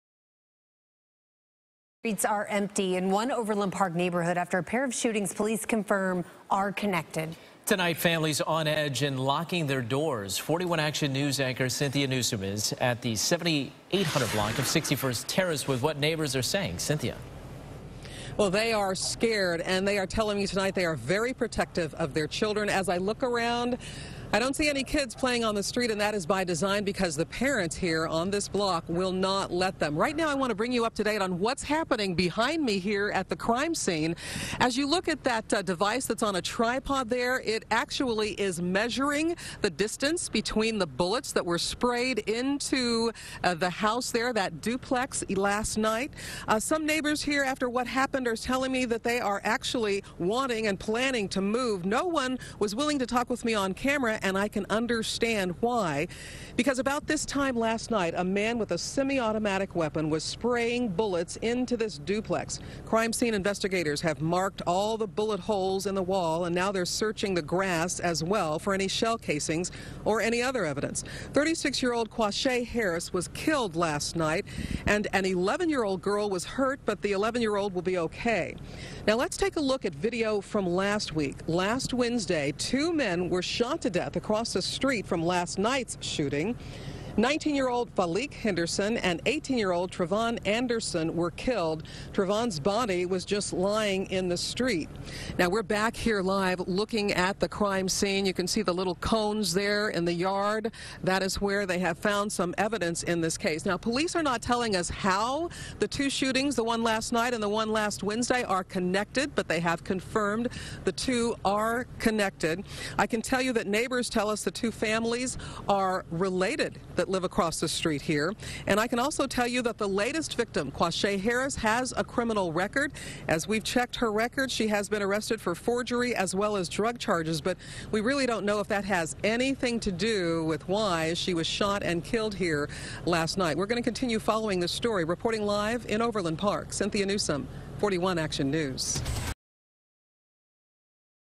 One woman dead, one child injured in Overland Park shooting, suspect on the run
The child is around 12 or 13 years old, according to police. The woman who died was in her mid-thirties.

41 Action News, KSHB, brings you the latest news, weather and investigative reports from both sides of the state line.